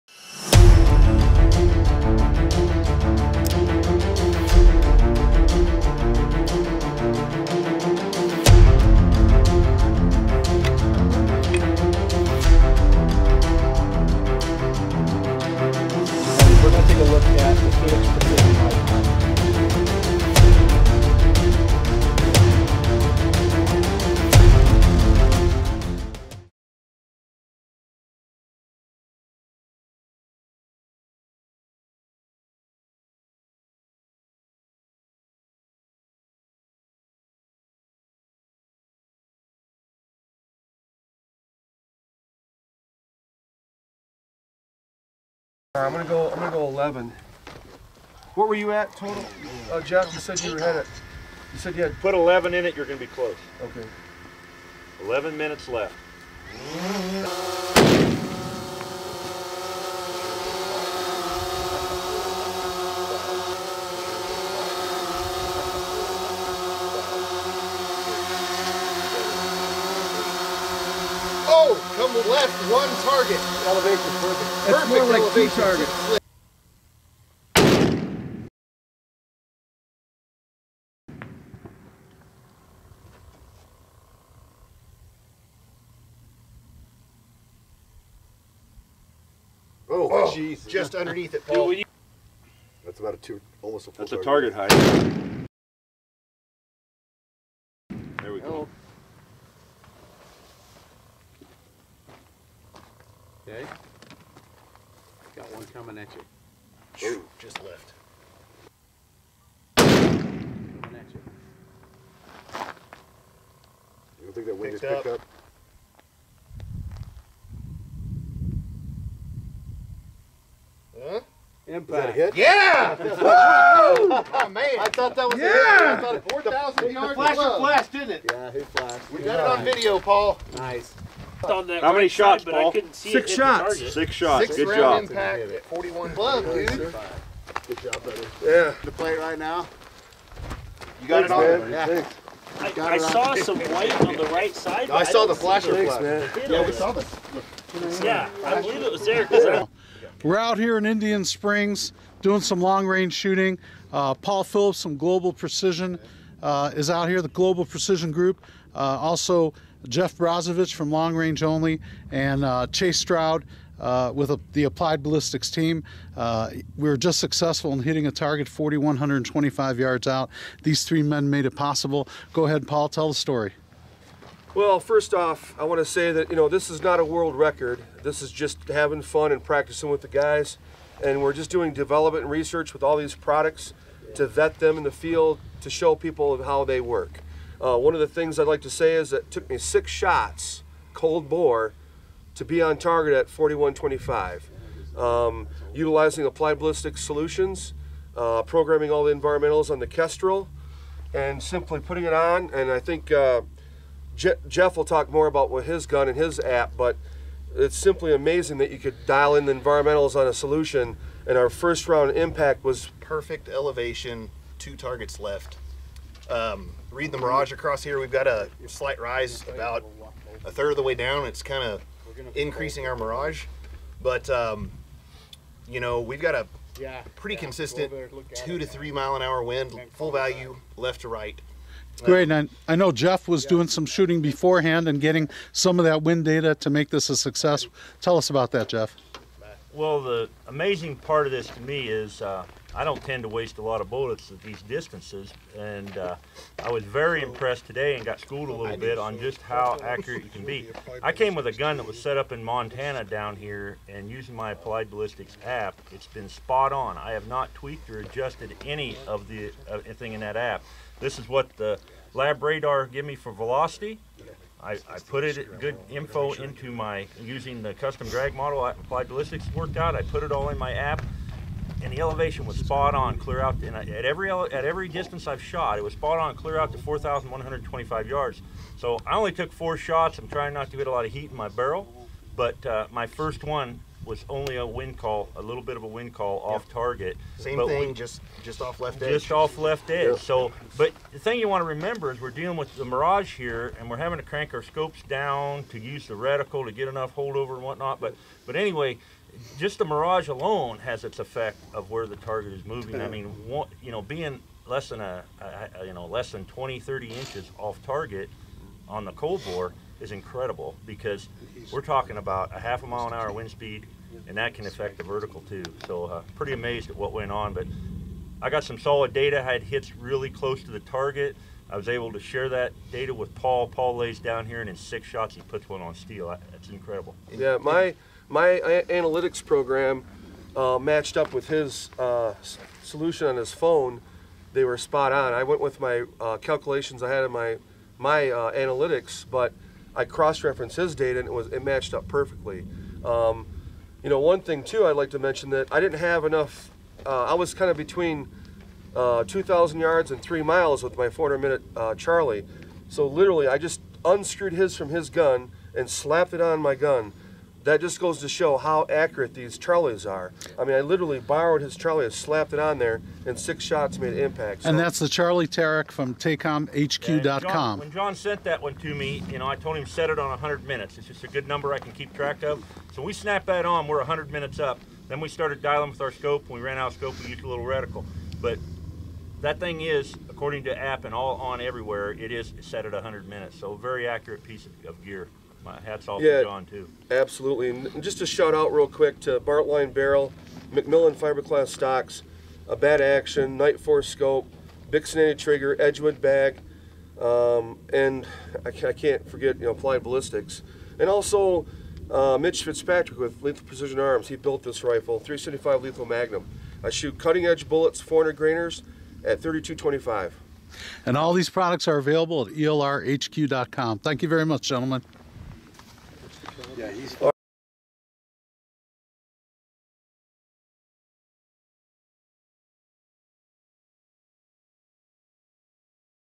Paul Phillips of Global Precision Group 4,125 yard shot
ELRHQ.COM participated in Paul Phillips 4,125 yard 6th round cold bore impact at Indian Springs on Jan 22, 2018.  Paul Phillips of Global Precision Group, is an ELRHQ.COM Champion, and member of Team McMillan.